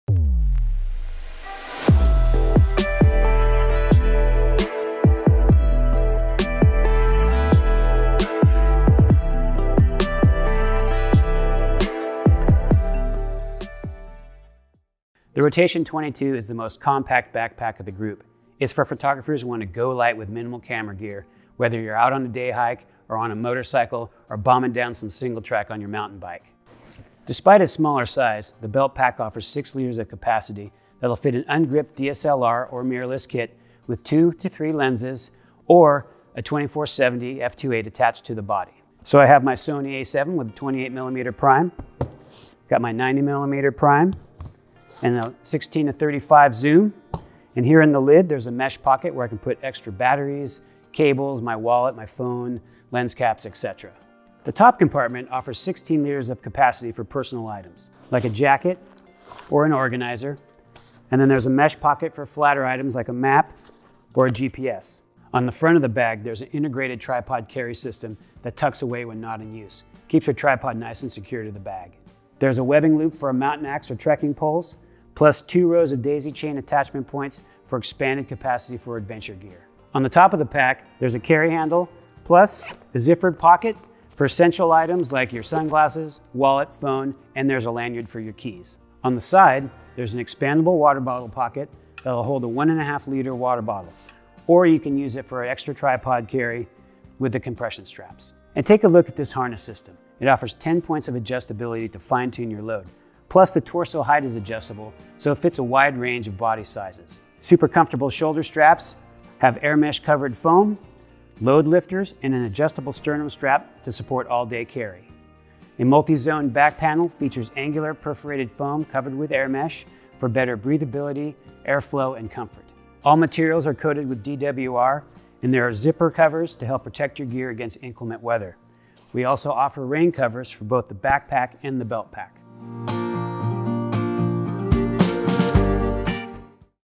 Rotation 22L Camera Backpack
Rotation180® technology enables photographers to quickly access their camera without taking off the backpack.

Simply rotate the integrated belt pack to the front of your body and your camera is at your fingertips. Snap the shot, slide the belt pack back in, and you’re on your way!